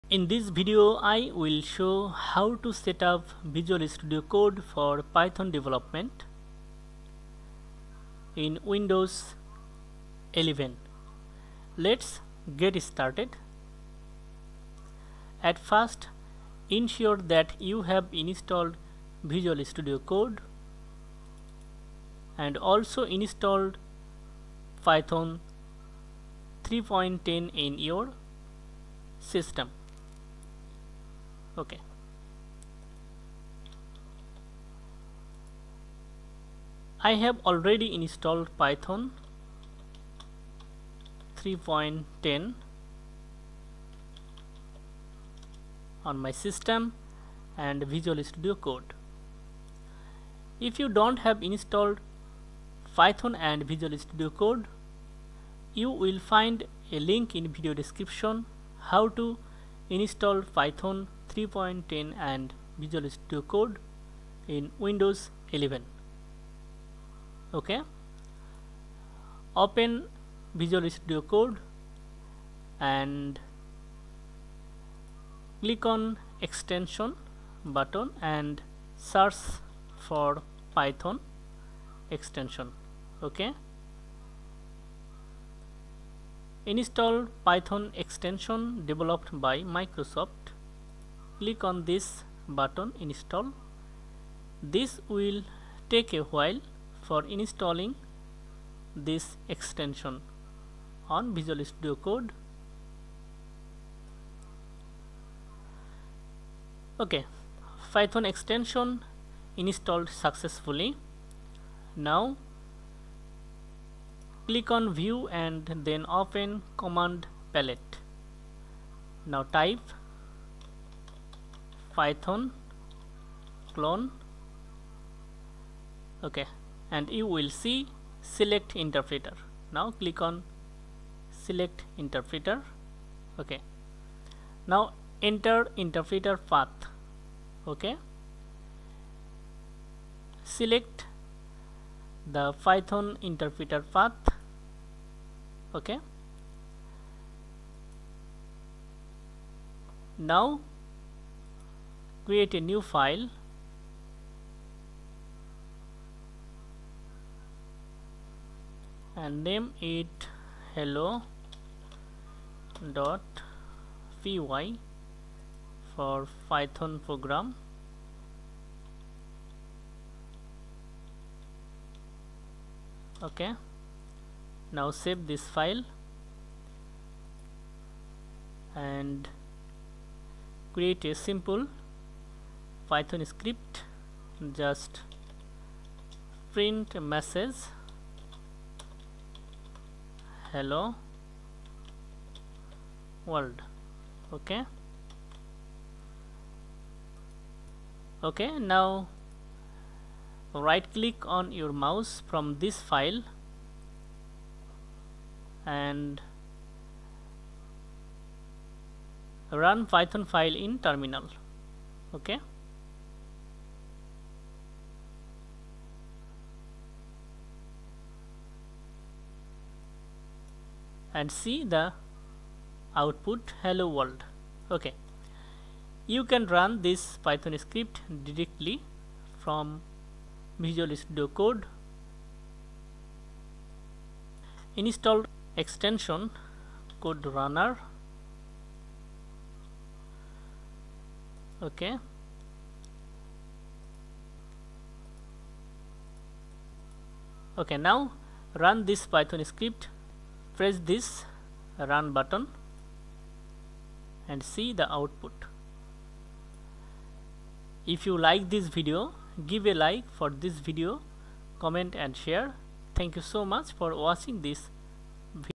How to setup Visual Studio Code for Python 3.10 development in Windows 11
Learn how to setup Visual Studio Code for Python 3.10 development in Windows 11